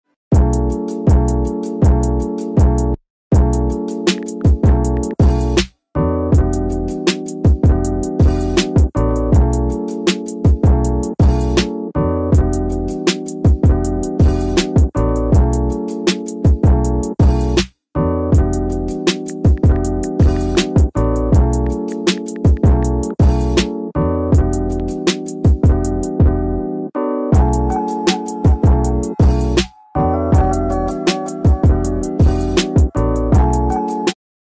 DIY rose water toner
A soothing rose toner perfect for your skin.